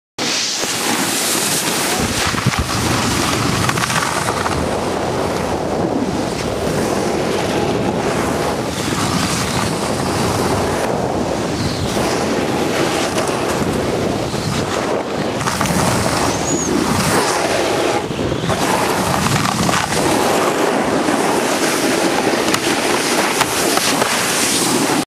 Follow cam på Even
En bra dag på Geilo vi filmet :)